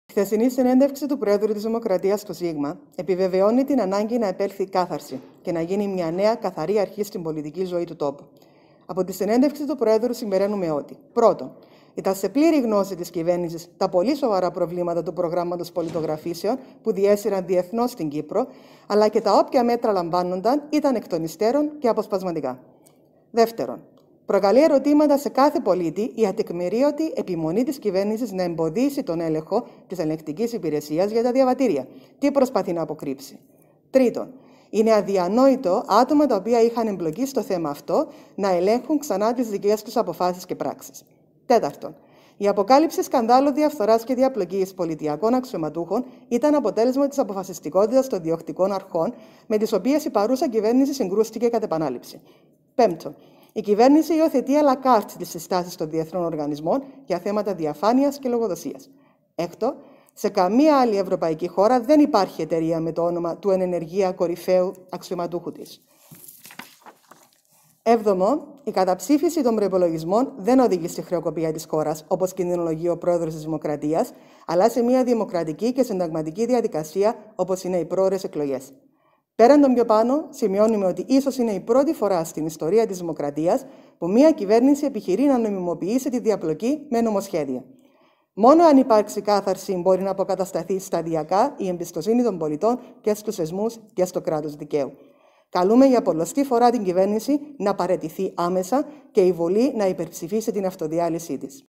Η κ. Ξάνθου για τη συνέντευξη του Προέδρου της Δημοκρατίας
Η χθεσινή συνέντευξη του Προέδρου της Δημοκρατίας στο Σίγμα επιβεβαιώνει την ανάγκη να επέλθει κάθαρση και να γίνει μια νέα καθαρή αρχή στην πολιτική ζωή του τόπου αναφέρει σε δηλώσεις της η αναπληρώτρια Πρόεδρος του Κινήματος κ. Έφη Ξάνθου.